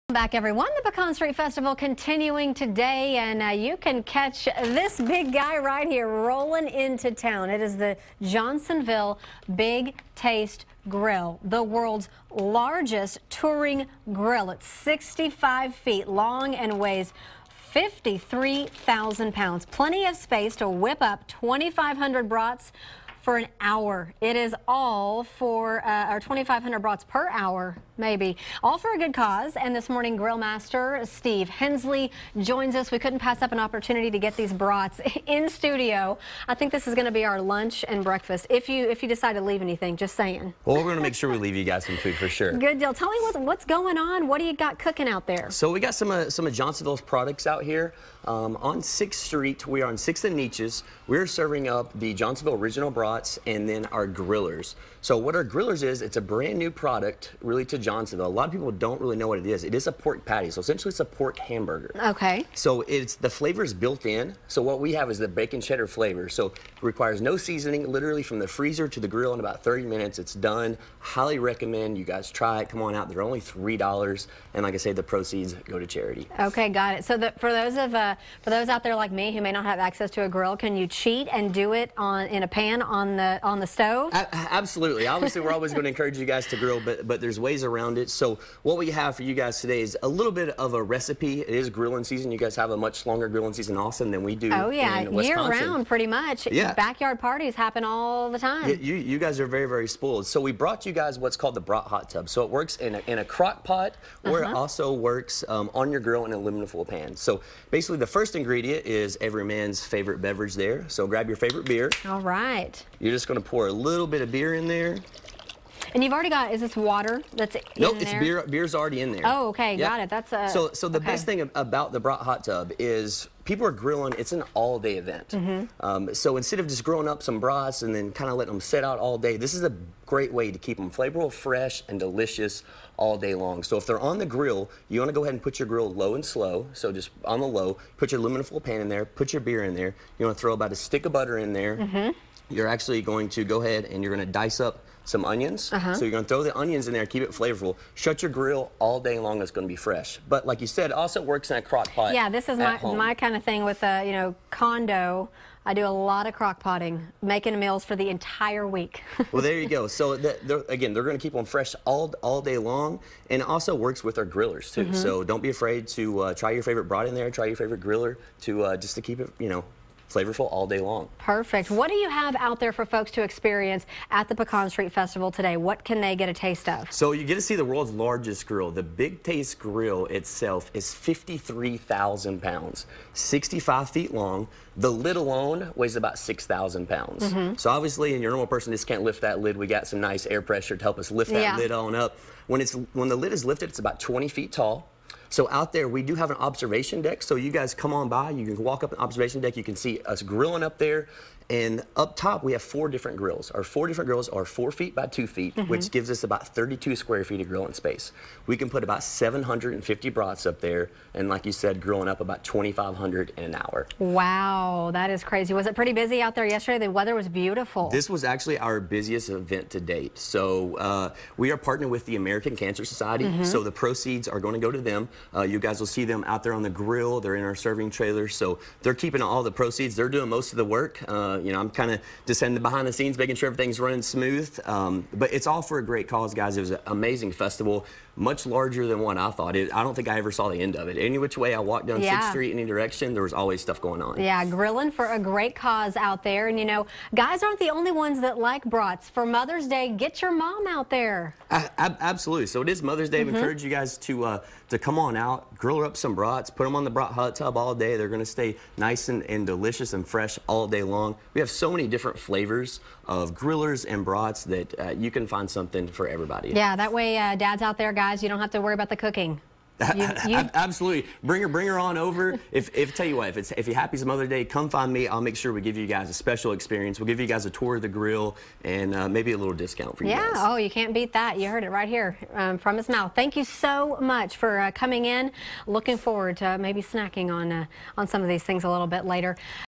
Johnsonville Big Taste Grill
Grillmaster Steve Hensley joins Erin Cargile in the studio to show us what's cooking in the world's largest touring grill.